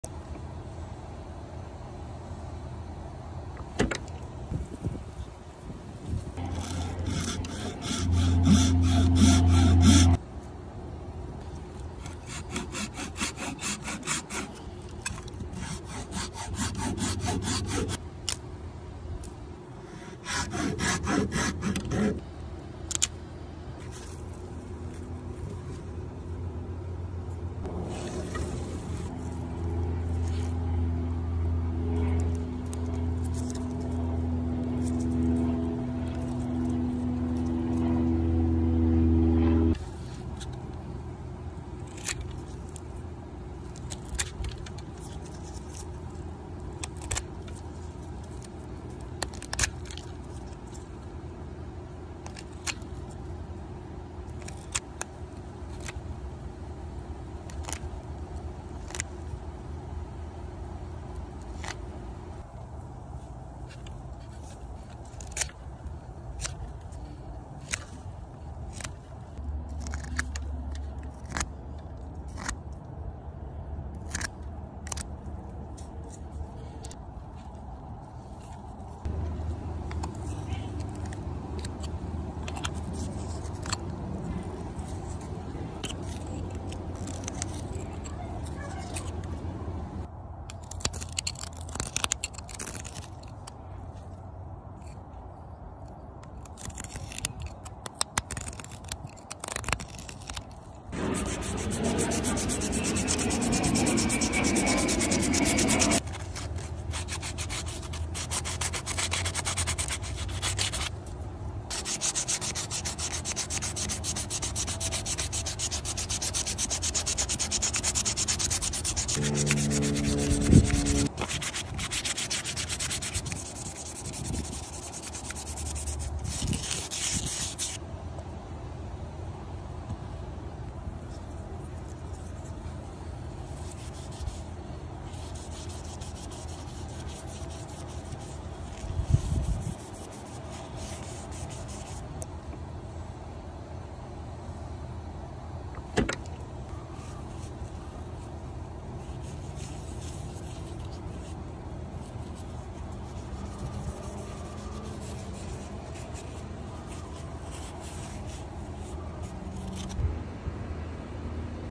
Wood Carving ASMR: Oakwood Spoon
Long form version of my shorts video with no voiceover. Just testing the waters with this one and seeing if you like the content. Enjoy (: